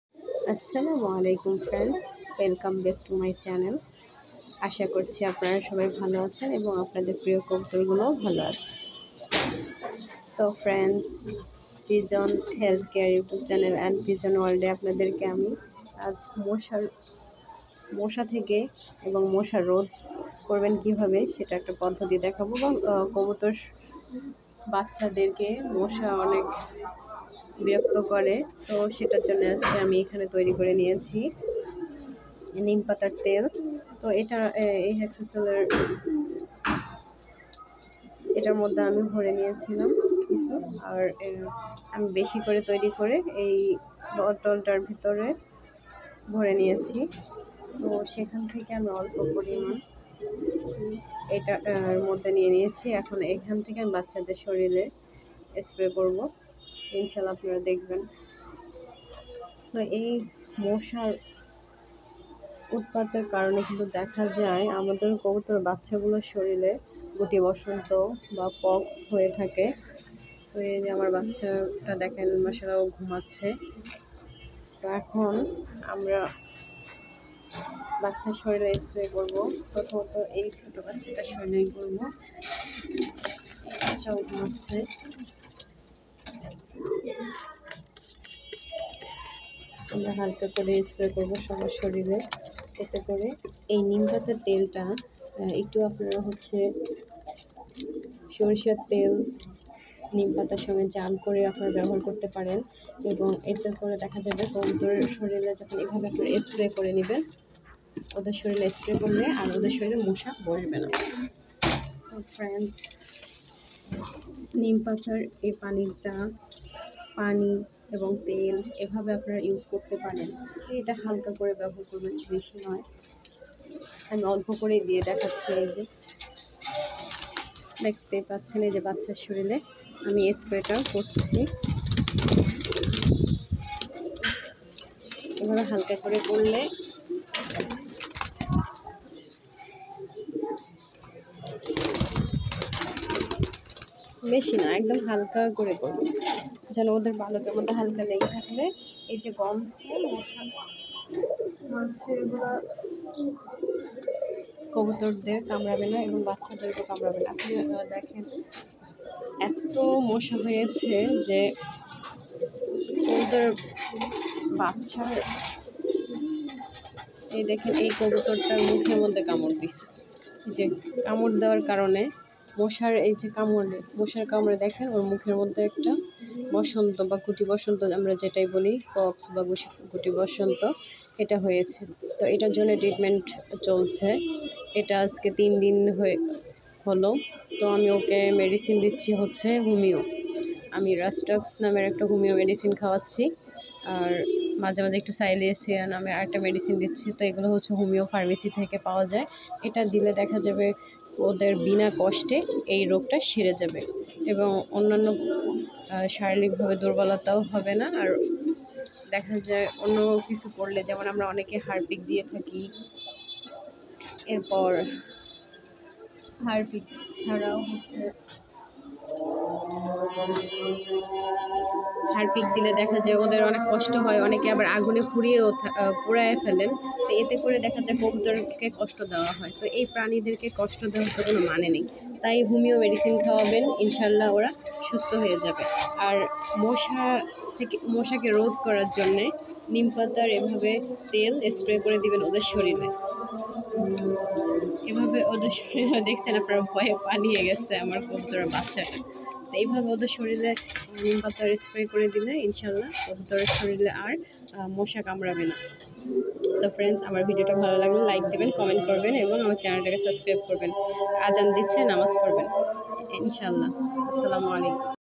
কবুতরের যত্নে নিম পাতার তেল দিয়ে মশা রোধ || মশা রোধ করতে নিম পাতার তেল || pigeon health care
কবুতরের যত্নে নিম পাতার তেল দিয়ে মশা রোধ || মশা রোধ করতে নিম পাতার তেল || pigeon health care # kobutor Palon pigeon health trips pigeon treatment

আসসালামু আলাইকুম , ফ্রেন্ডস
আজ আমরা কবুতরের স্থান থেকে মশা রোধ করার বা কবুতকে মশার কামড় থেকে রক্ষা করতে নিম পাতার তেল ব্যবহার করব । নিম পাতার তেল ব্যবহার করলে,  মশা কবুতরের শরীরে কামড় দিবে না এবং মশার কামড়ে যে রোগ গুলো হয়ে থাকে যেমন - কবুতরের বাচ্চার গুটি, গুটি বসন্ত, পক্স  এধরনের রোগ দেখা দেয় । কবুতরের বাচ্চার গুটিবসন্ত ও পক্স থেকে রক্ষা করতে নিম পাতার তেল ব্যবহার করুন, ইনশাআল্লাহ। কবুতরের বাচ্চা মশা কামড় থেকে রক্ষা পাবে।